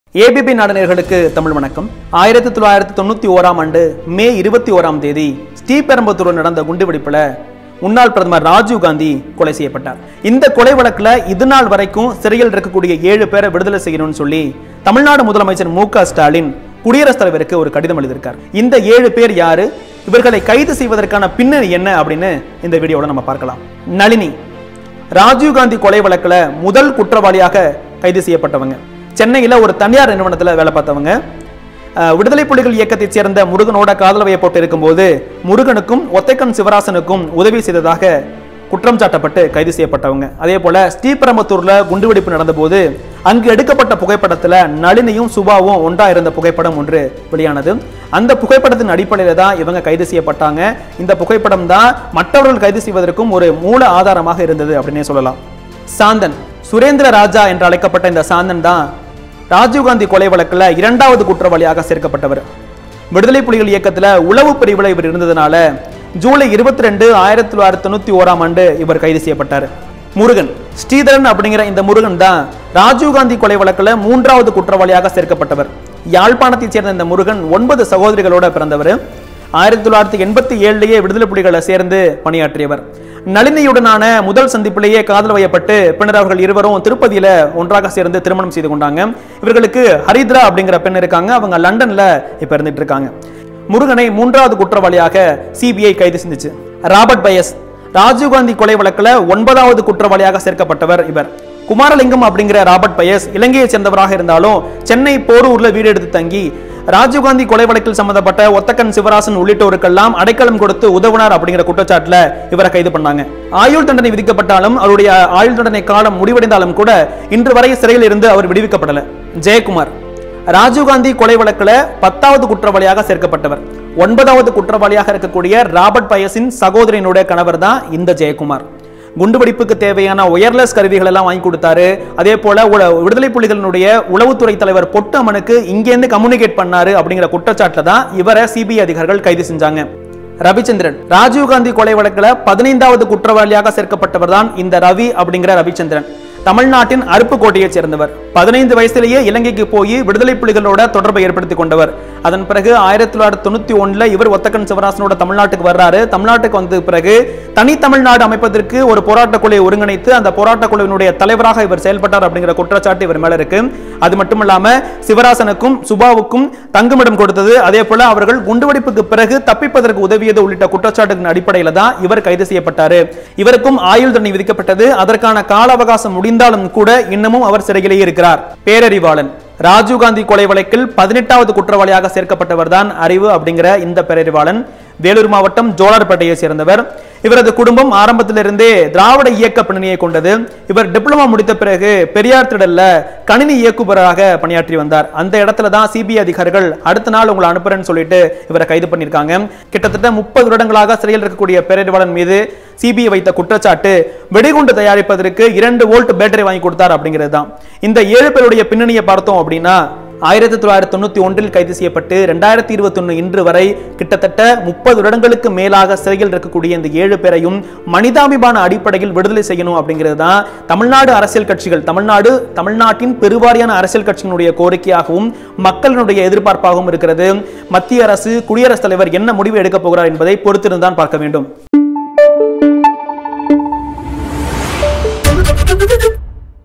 Rajiv Gandhi Assassination Case Convicts Details | யார் அந்த ஏழு பேர்? | Perarivalan | Nalini | ABP
Rajiv Gandhi Assassination Case Convicts Details | யார் அந்த ஏழு பேர்? | Perarivalan | Nalini | Santhan | Jayakumar | Ravichandran | Murugan

The court convicted and sentenced the seven persons who facilitated the assassination of Rajiv Gandhi to life imprisonment.
They are currently undergoing life imprisonment in prisons in India. They are:
Murugan alias Sriharan - A LTTE operative from Sri Lanka
Nalini - Wife of Murugan.
Nalini is a citizen of India.
Santhan alias T. Suthenthiraraja - A Sri Lankan national.
Robert Pious - A Sri Lankan national.
Jayakumar - The brother in law of Robert Pious.
Ravichandran - A Sri Lankan national.
A. G. Perarivalan - An Indian citizen who was arrested for supplying a 9-volt battery for the explosive device.